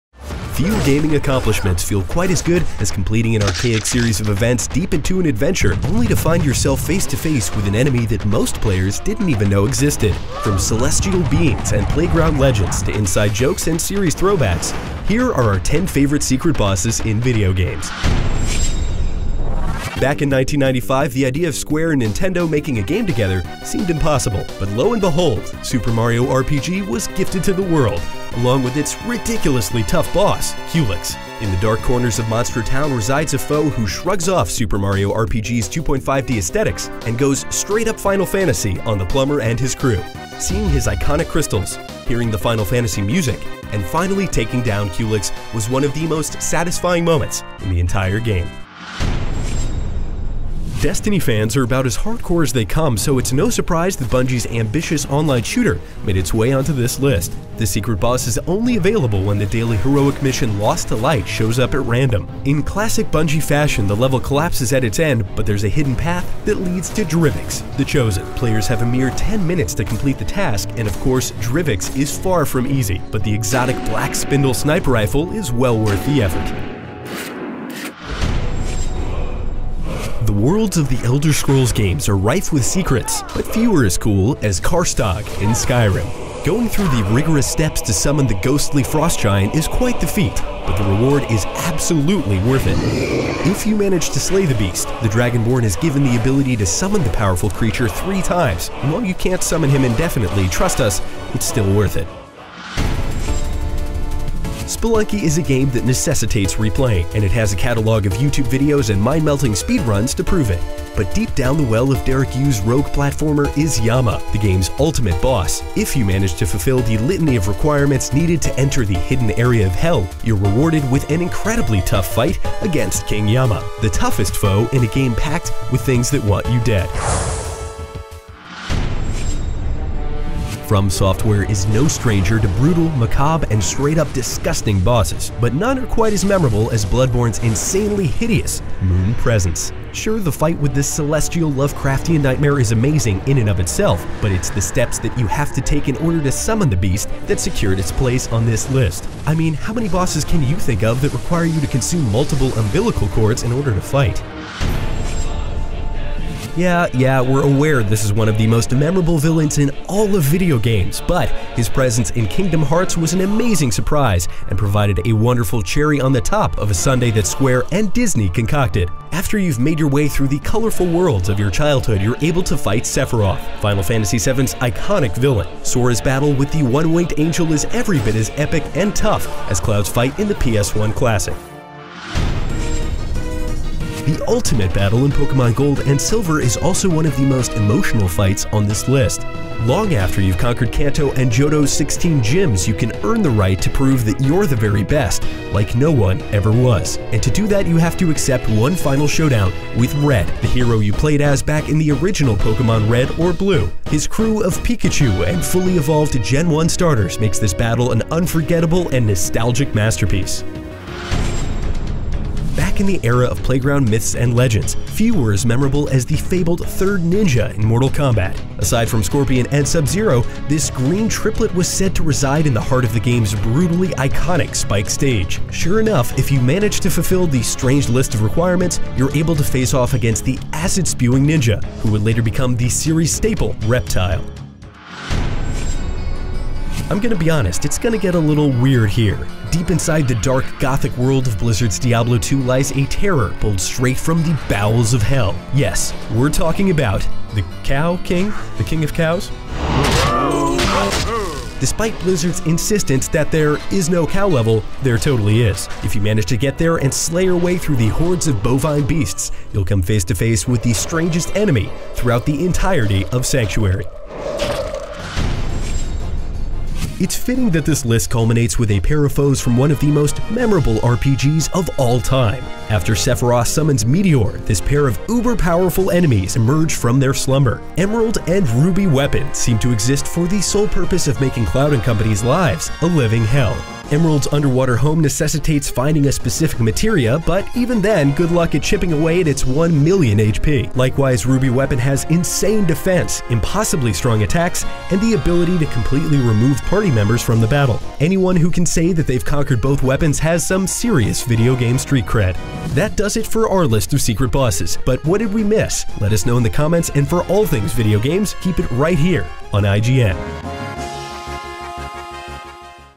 Top 10 Secret Bosses in Games
We count down the most mysterious and elusive bosses in gaming!

Follow IGN for more!